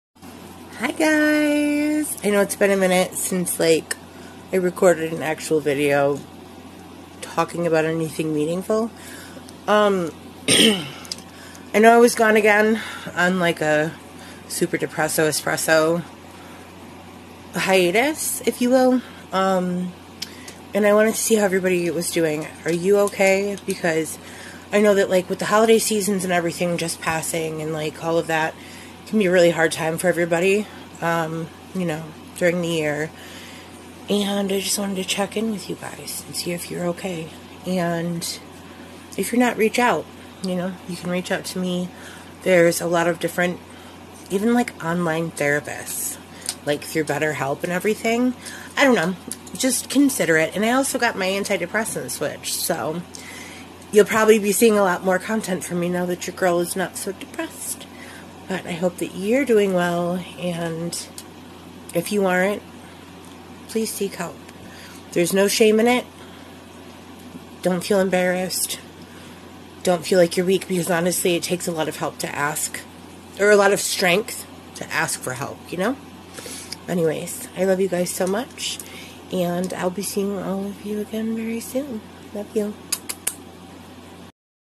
mental health struggles are no joke, and i just want you to know youre not alone. | Community | Love
Are you ok bestie? Mental health struggles are no joke.. Plz don't suffer in silence. ❤ I love you, and I promise.. This too shall pass. 🤟🏼